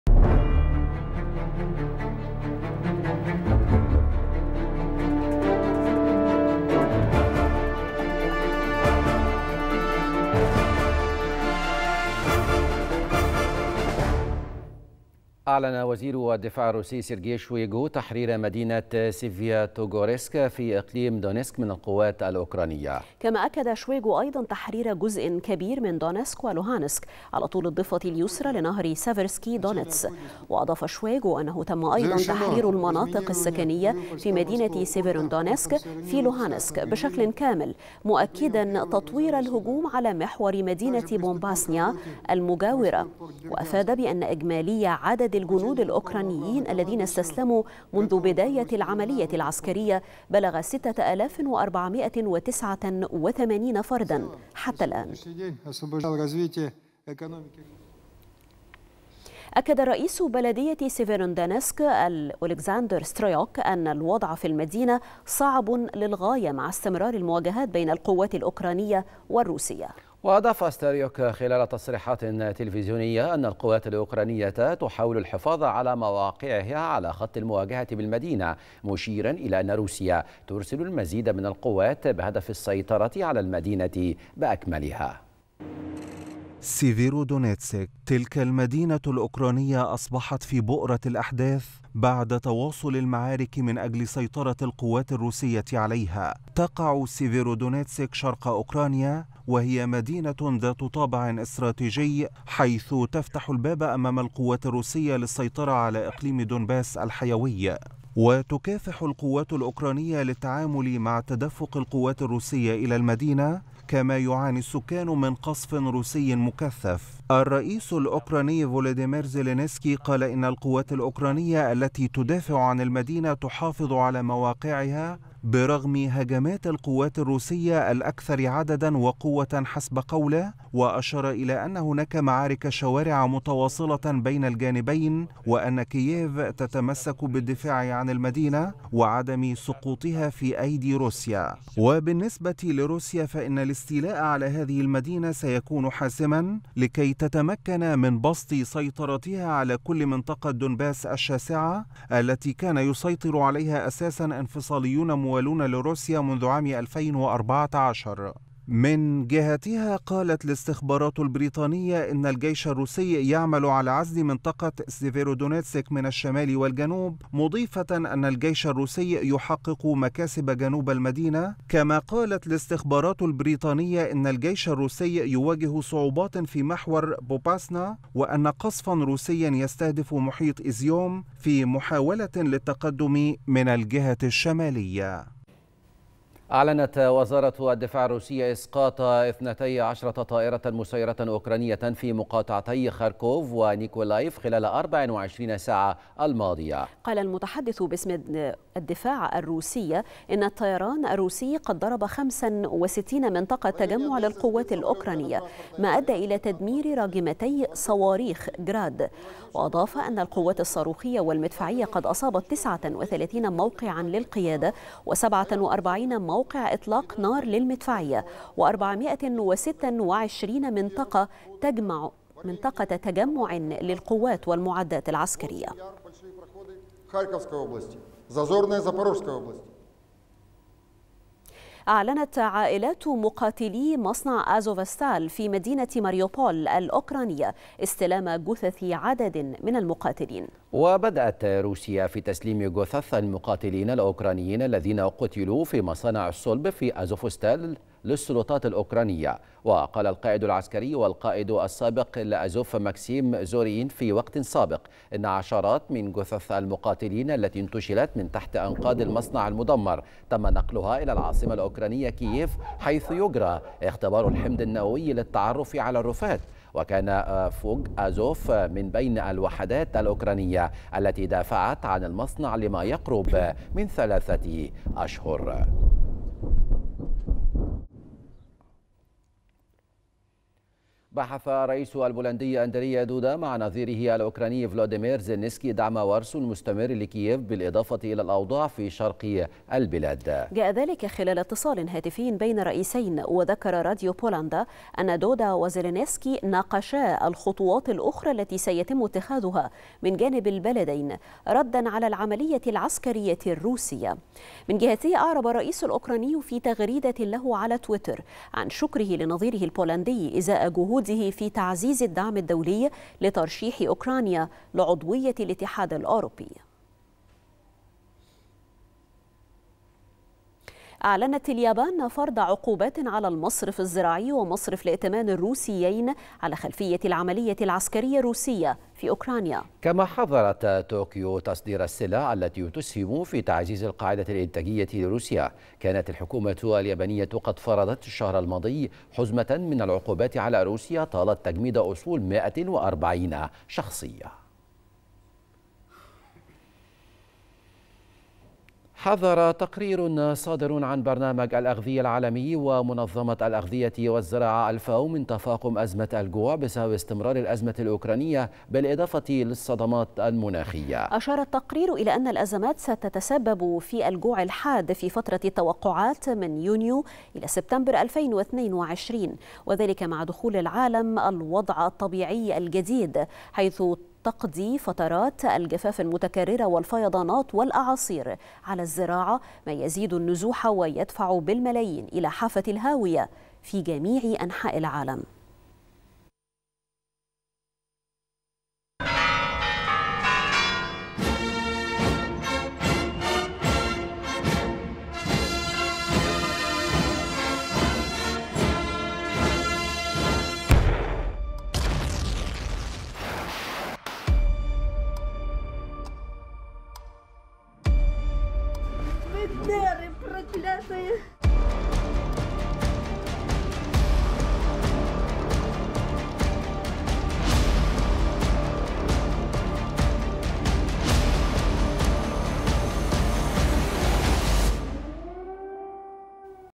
روسيا تعلن تحرير جزء كبير من دونيتسك ولوهانسك من القوات الأوكرانية.. وأهم أخبار الأزمة الأوكرانية
نشرة الثالثة | روسيا تعلن تحرير جزء كبير من دونيتسك ولوهانسك من القوات الأوكرانية.. وأهم أخبار الأزمة الأوكرانية